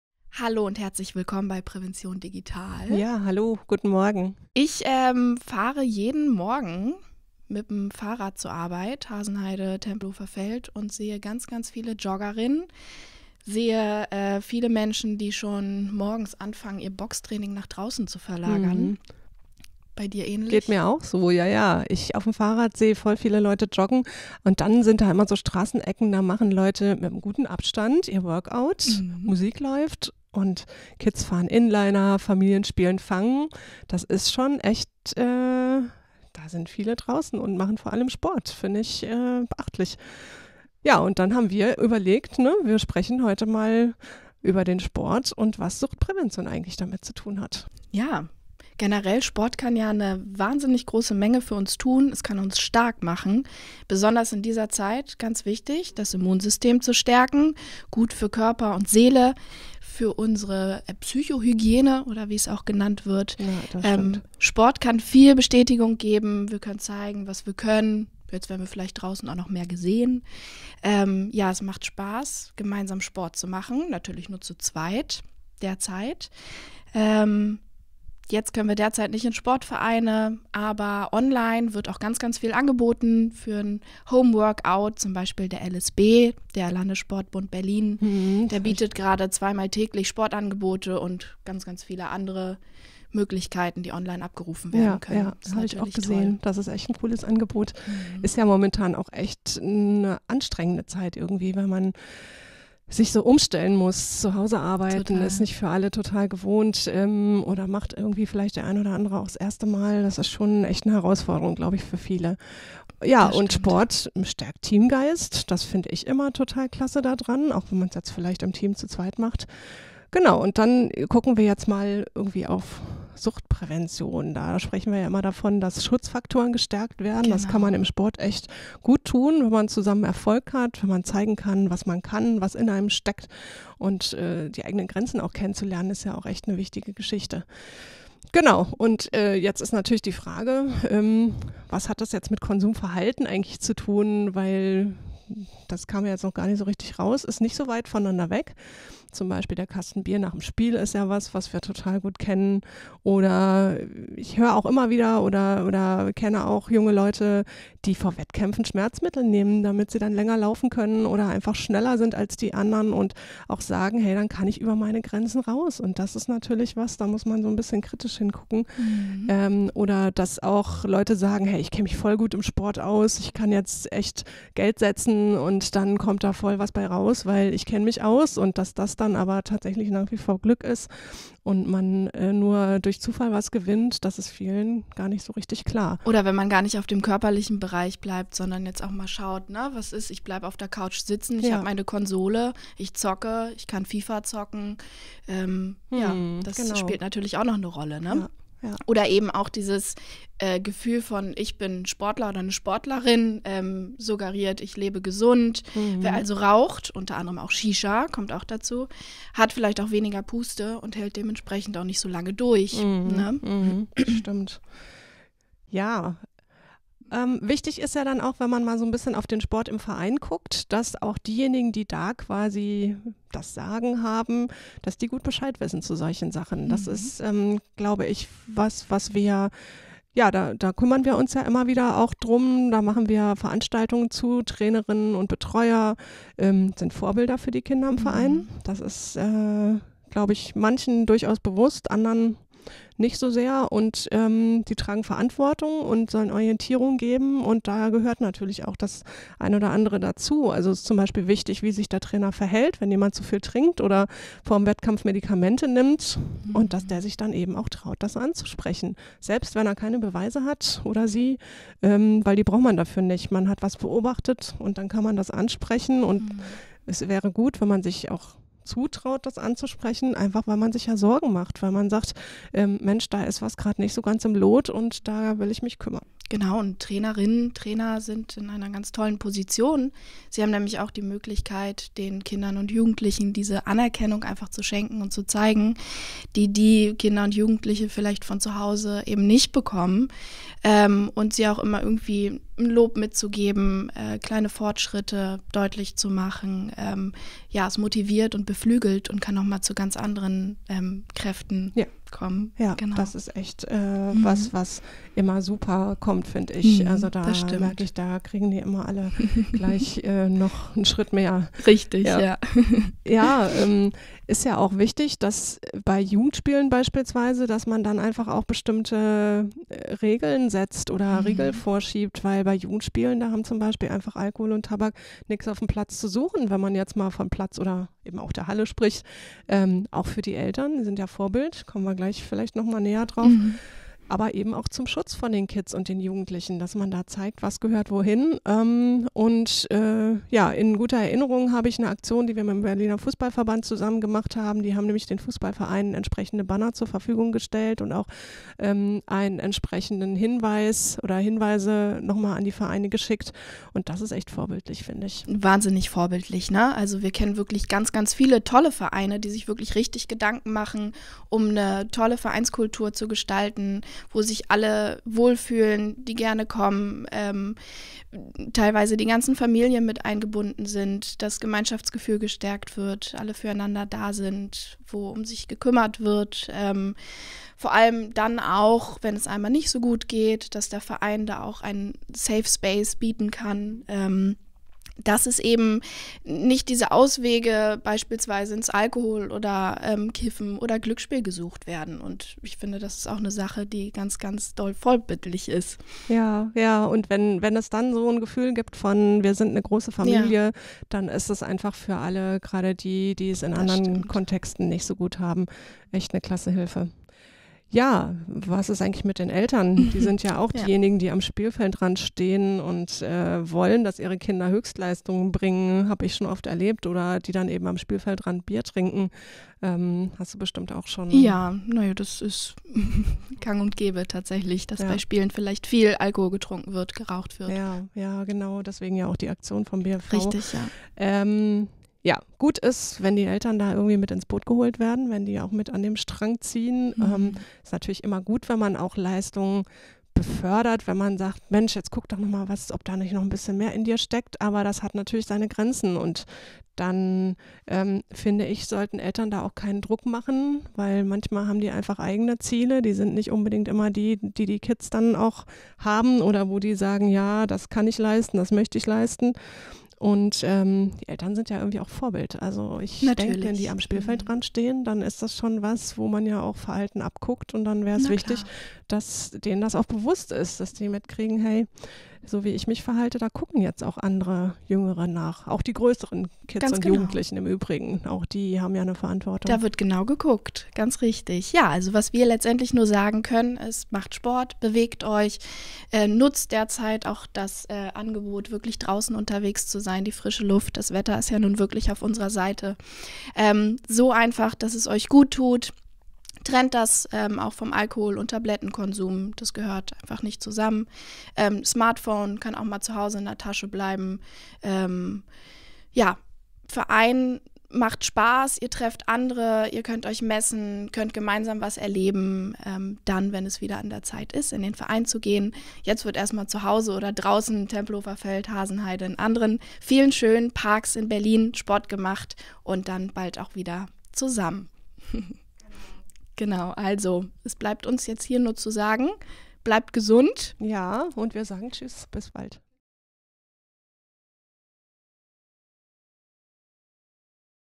Podcast: Was hat Sport mit Suchtprävention zu tun?
Sport kann eine Menge für uns tun, kann uns stark machen – gerade in dieser Zeit. Wie genau das mit Suchtprävention zusammenhängt und was Sportler, Sportvereine und auch Eltern beitragen können, besprechen unsere Expertinnen im heutigen Podcast.